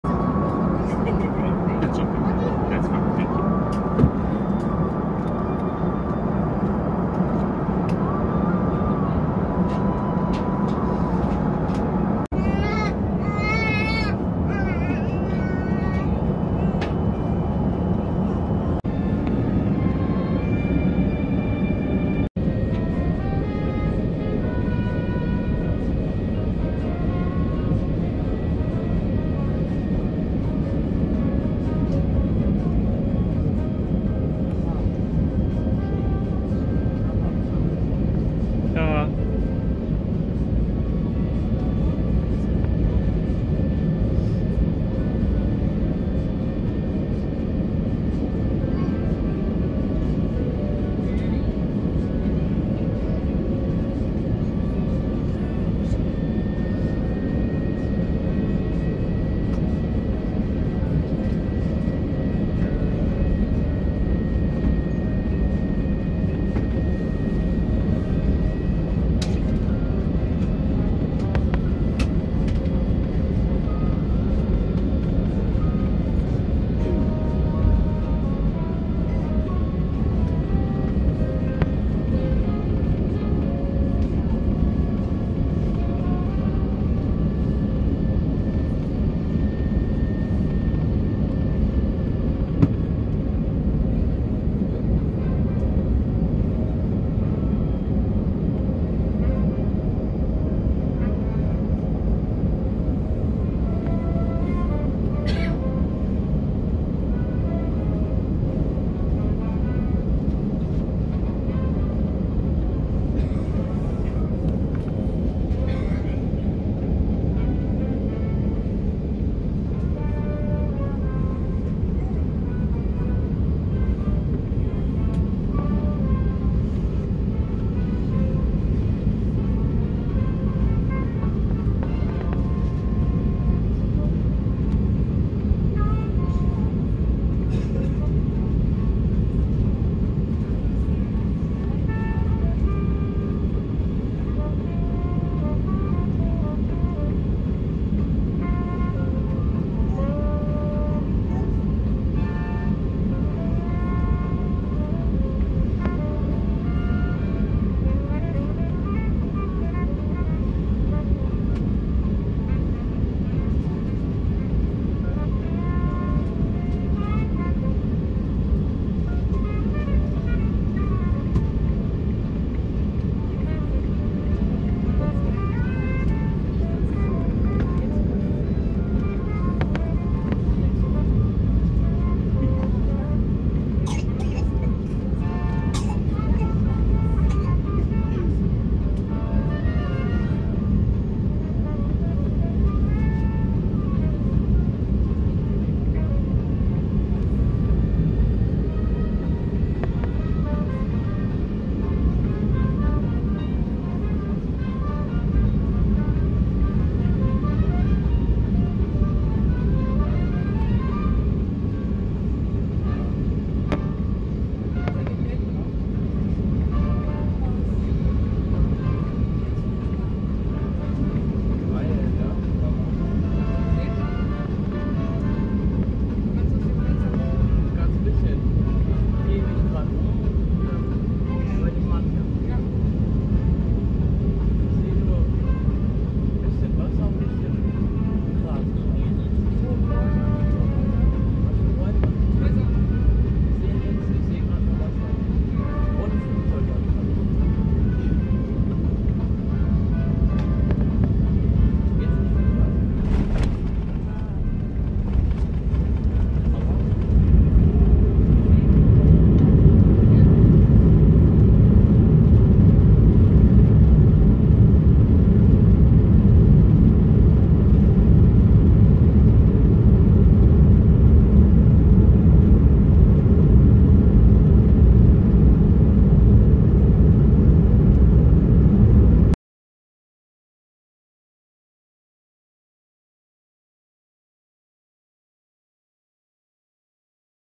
Full approach and landing New York JFK in Singapore Airlines Airbus A380 (9V-SKF)
Landing on runway 4R during sunny weather in the oldest Airbus A380 of SIngapore airlines (9V-SKF). Nice view on Long Island. Nearly no background noise, very quite. Very smooth landing. You nearly hear no engine sounds demonstrating how quite A380 is.

Anflug und Landung auf dem Flughafen JFK in New York im Singapur Airlines Airbus A380. Tolle Sicht auf Long Island und New York, bei sehr gutem Wetter.